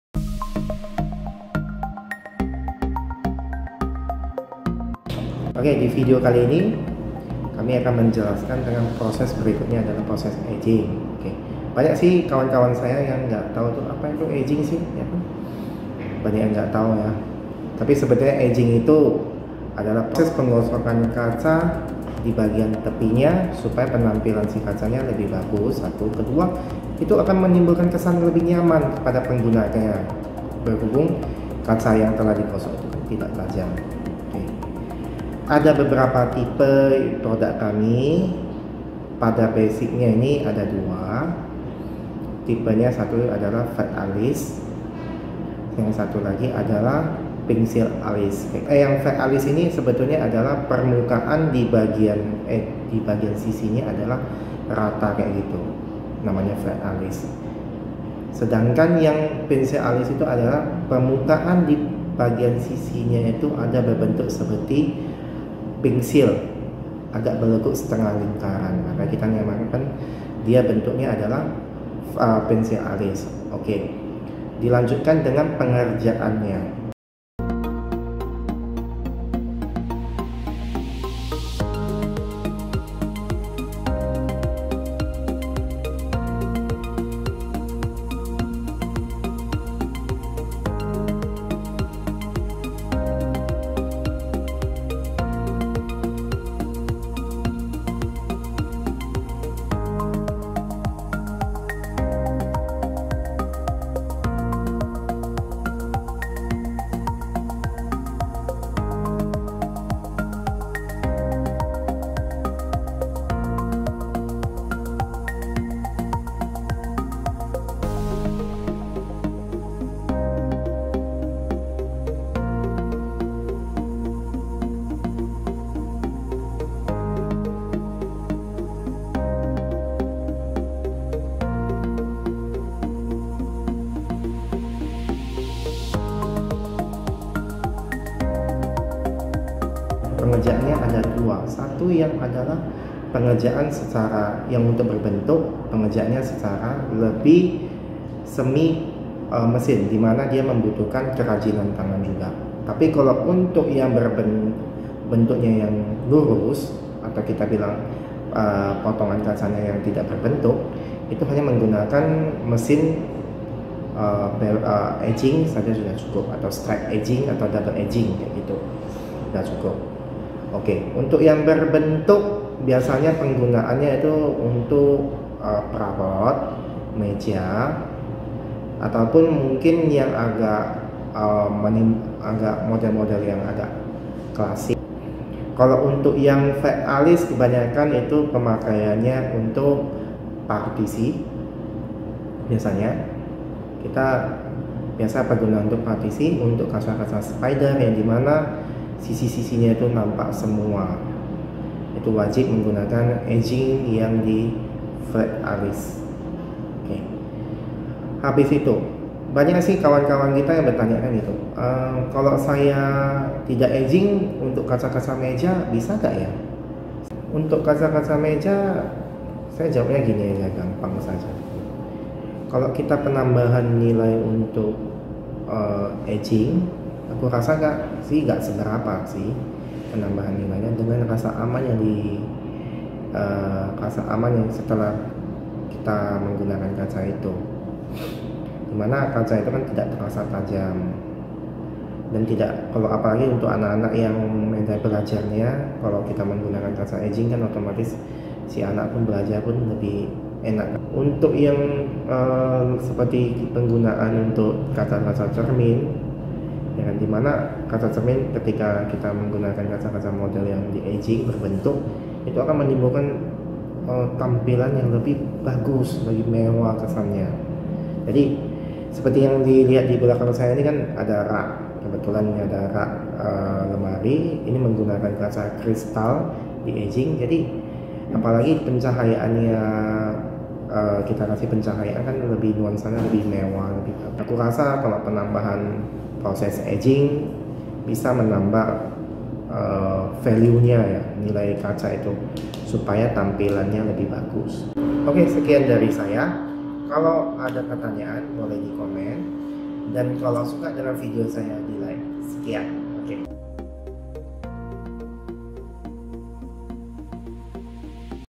Proses Edging LURUS dan BENTUK pada Kaca (Agar Kaca AMAN dan terlihat MEWAH)
Proses edging merupakan salah satu proses penting pada kaca. Edging dilakukan dengan cara menggosok tepian kaca agar lebih aman dan tidak tajam. Edging juga dapat dilakukan untuk meningkatkan sifat dekoratif pada kaca sehingga menciptakan kesan mewah. Proses edging wajib dilakukan untuk setiap produk kaca seperti untuk kebutuhan interior (partisi hingga perabot) dan untuk kebutuhan eksterior bangunan.